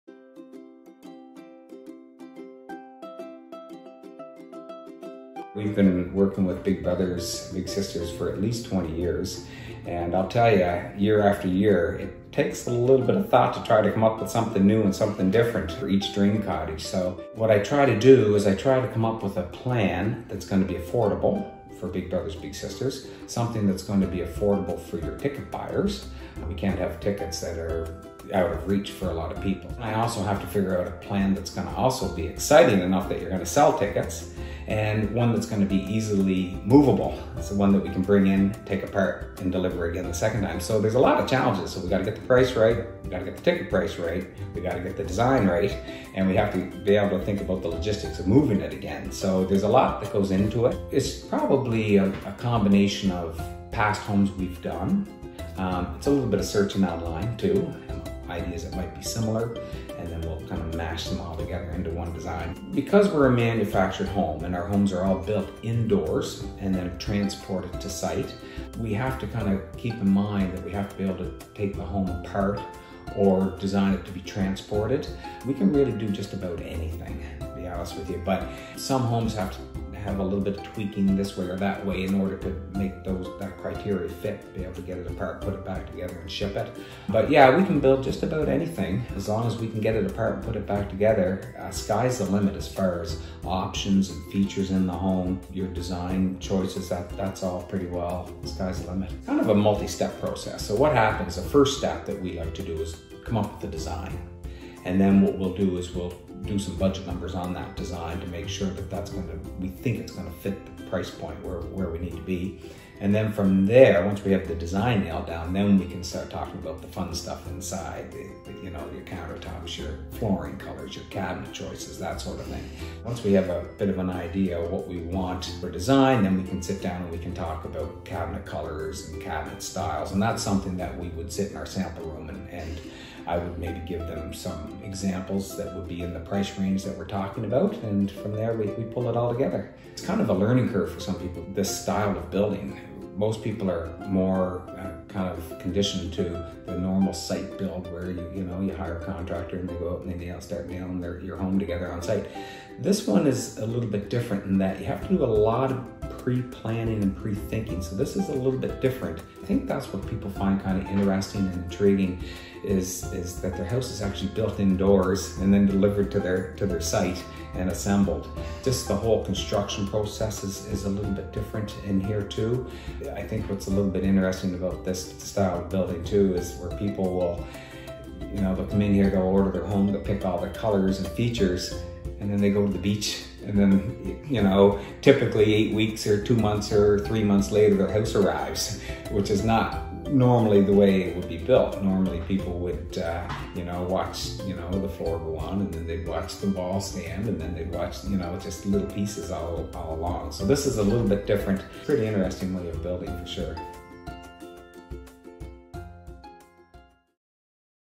Behind the Scenes at the Dream Cottage: How Rob Designs the Dream Cottage
Have you ever wondered what goes into designing the Dream Cottage? Find out what the challenges are, what happens after a floor plan is decided on and what makes a Prestige Home build unique.